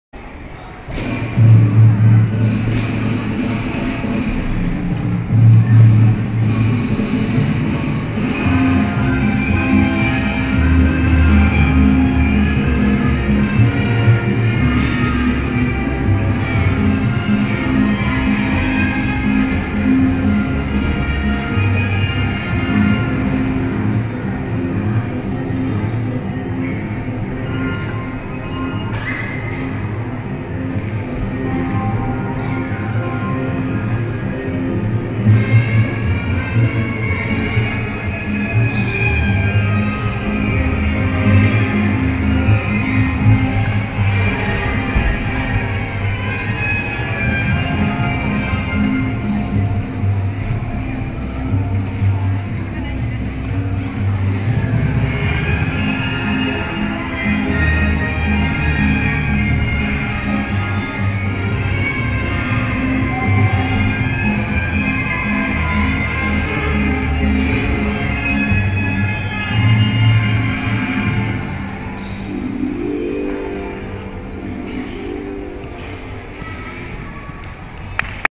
Siiri Saukkonen 2014 Finn junCh AA FX
Siiri Saukkonen, who represents the organizing team Kieppi, on floor at 2014 Finnish junior Championships AA. She qualified as 5t to the final with 3.9/11,70